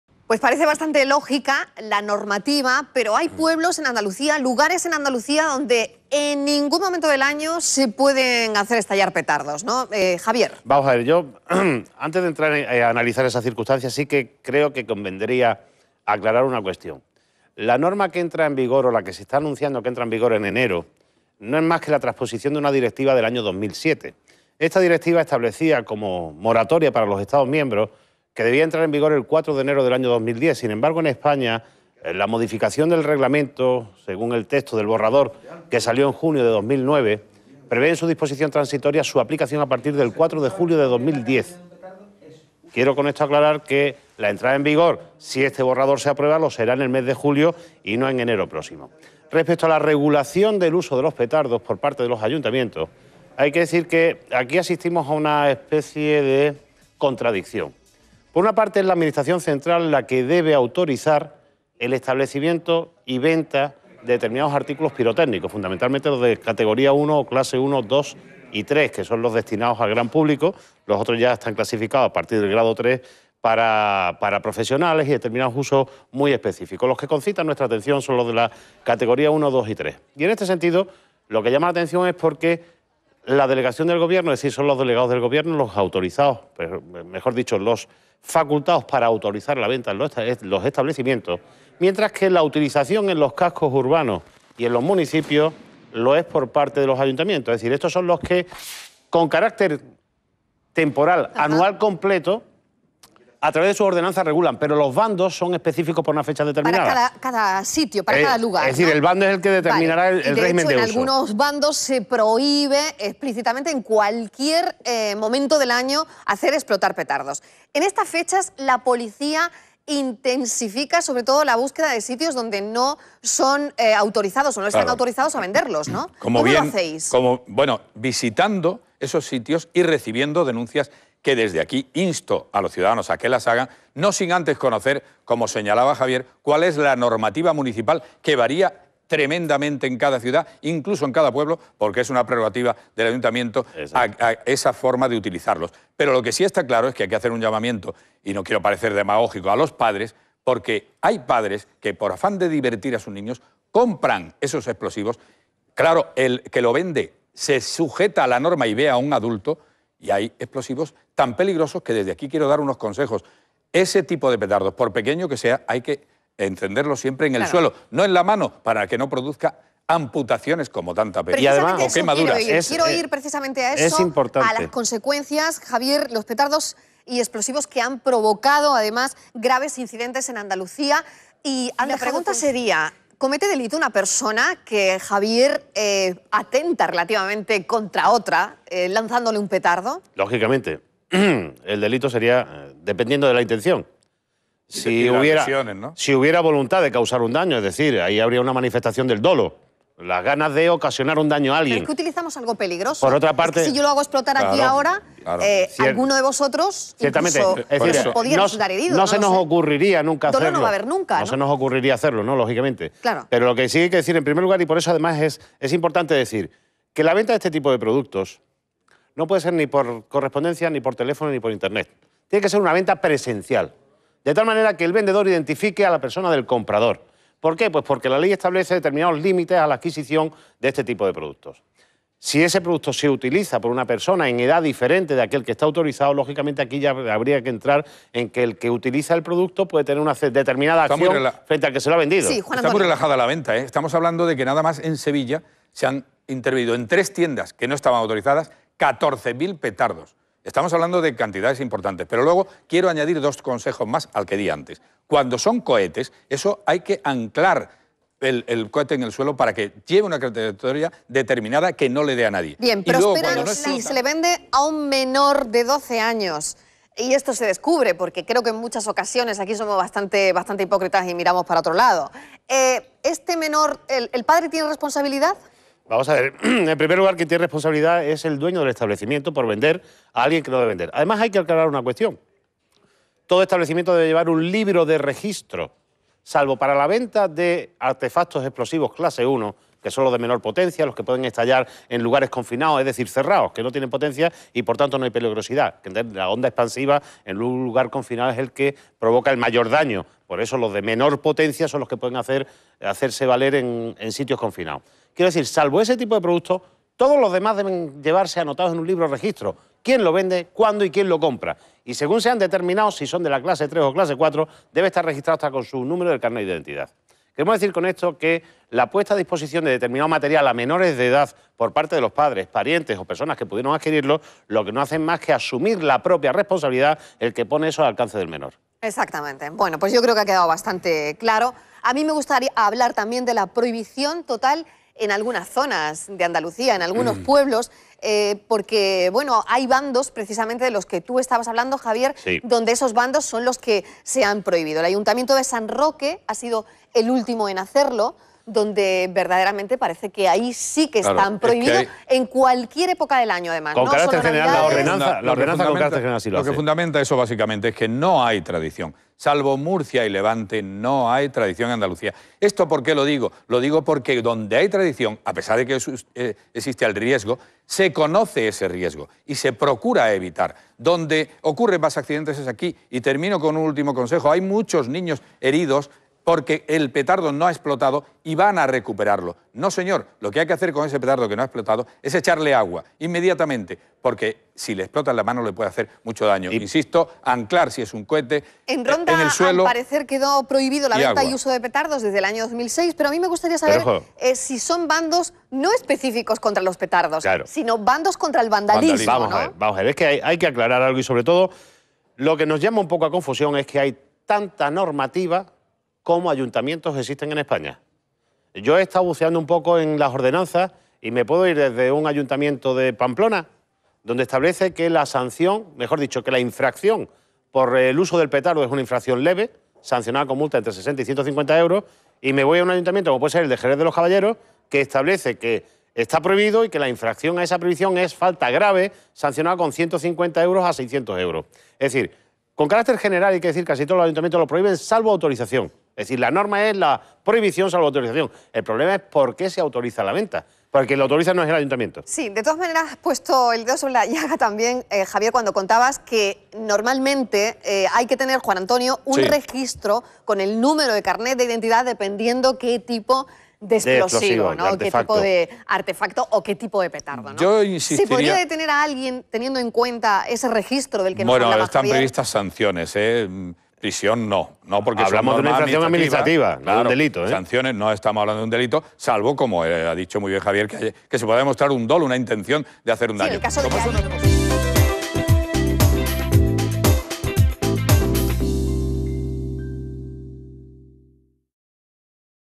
Nueva normativa Petardos Asesores Mira la Vida
Entra en vigor la nueva normativa que prohibe el acceso a petardos para menores de 12 años. Nuestros asesores nos cuentan las consecuencias legales del mal uso de esta forma de diversión.